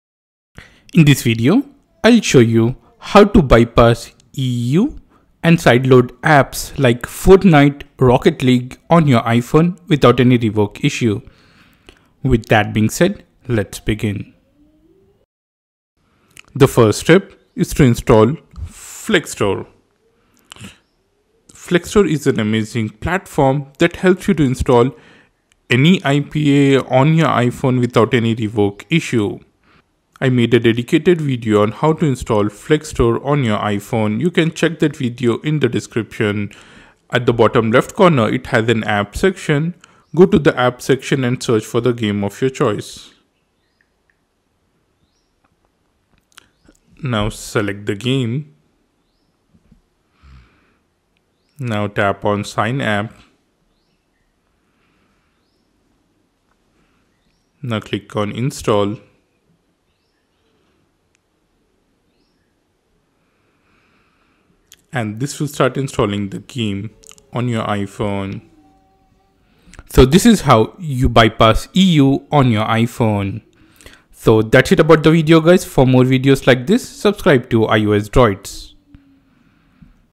How to Bypass EU Sideloading on iPhone or iPad: Easy Step-by-Step Guide
In this video, we'll show you how to bypass EU sideloading restrictions on your iPhone or iPad. With recent regulations in place, sideloading apps in the EU can be tricky, but our easy-to-follow guide will help you navigate this process safely and effectively. Whether you're looking to install third-party apps or customize your iOS device, this method works for you.